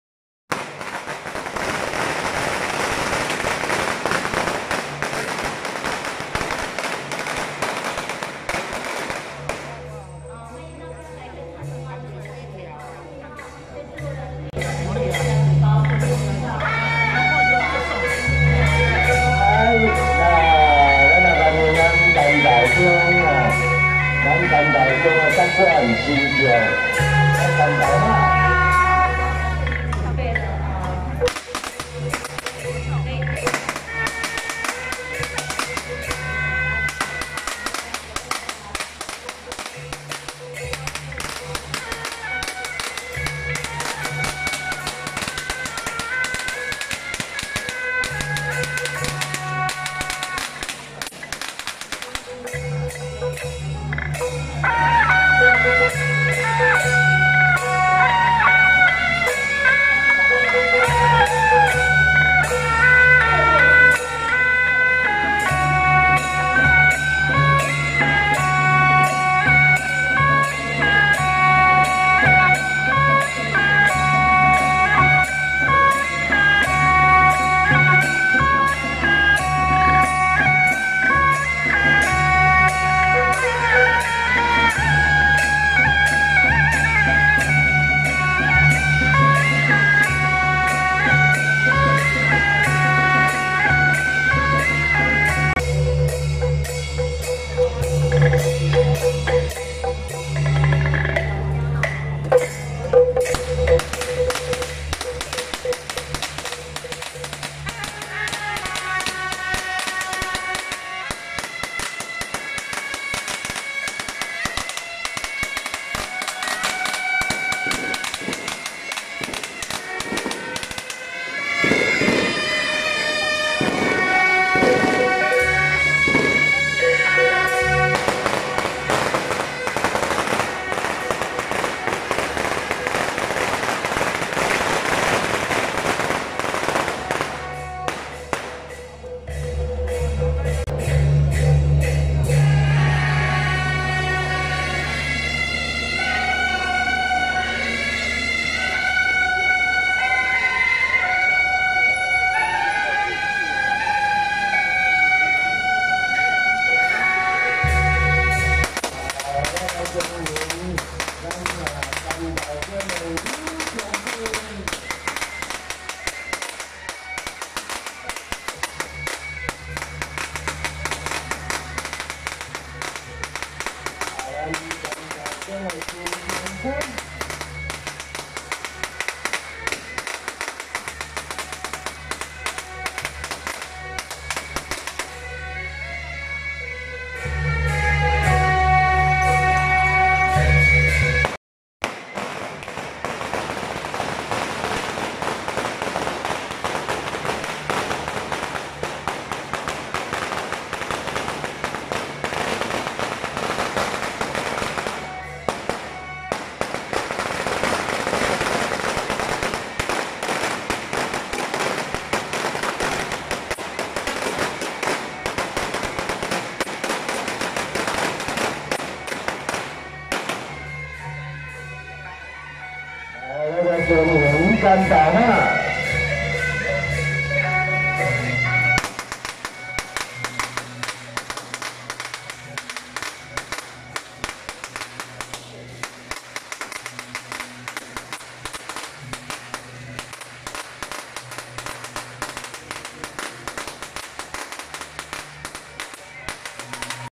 【關渡宮 慶贊 士林神農宮】~109士林神農宮建廟280週年暨鎮殿神農大帝首度出巡遶境
(0:00)關渡宮-鮮花頭旗車
(0:26)三芝協和軒-北管＆關渡宮-駕前金將軍,柳將軍
(3:12)關渡宮-天上聖母 聖駕